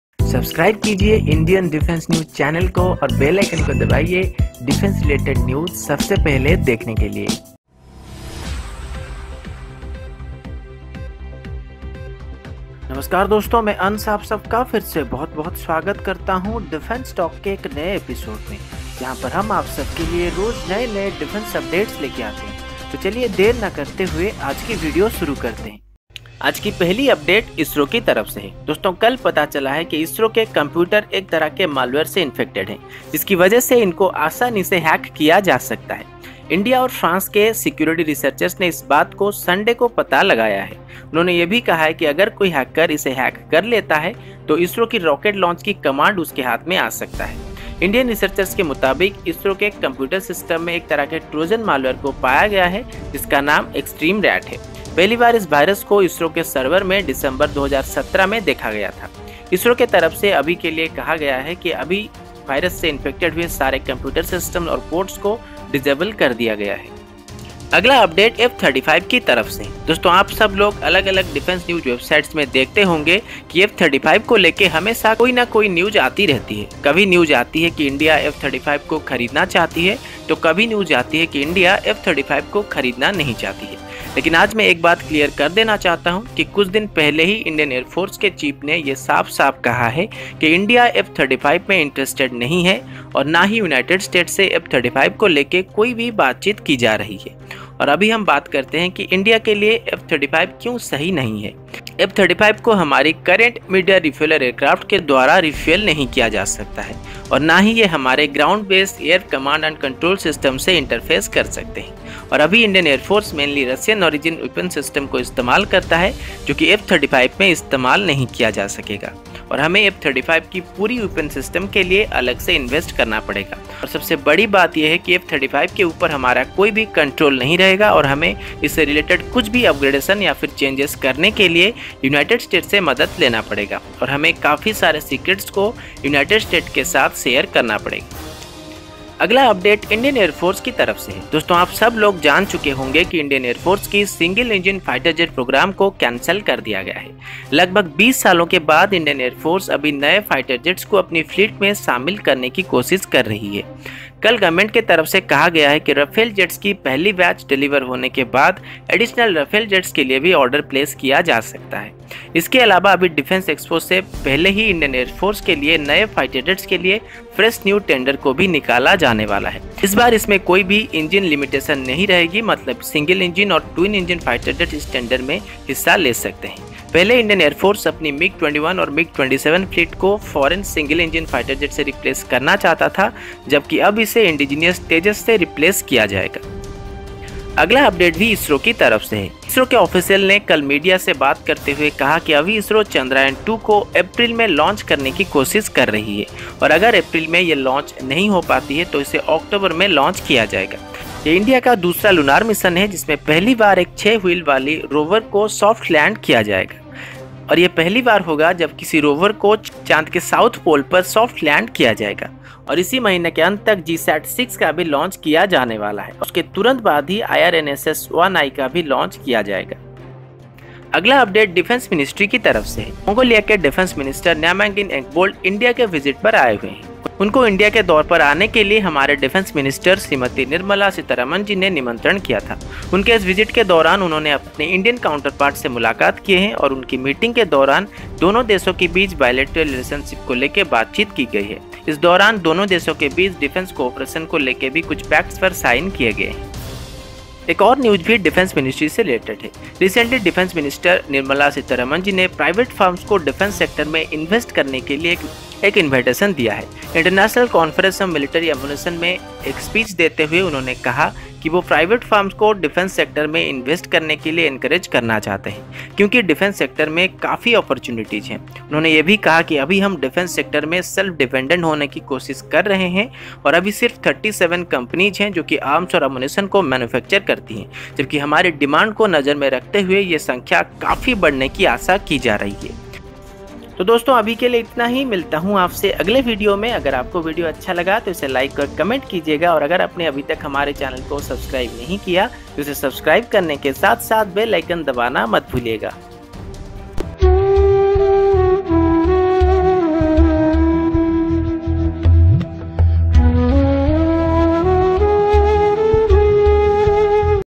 Indian Defence News:ISRO system Hacked,F-35 is not for IAF,Chandrayaan-2 in April/Oct,MMRCA 2,Hindi
Indian Defence News:ISRO system Hacked,F-35 is not for IAF,Chandrayaan-2 in April/Oct,MMRCA 2 before April 11,Hindi

Here is today's top Indian defence updates/Latest Indian defence news in hindi:-

headlines:--
--ISRO computer had malware, could’ve been hacked, say researchers

--Isro plans Chandrayaan-2’s rendezvous with moon in April

--F-35s are not the right choice for India’s defence | Opinion

--Mongolia, India discuss defense cooperation

--Nirmala Sitharaman Invites Private Sector To Invest In Defence

--IAF to embark on a long shopping sortie for a jet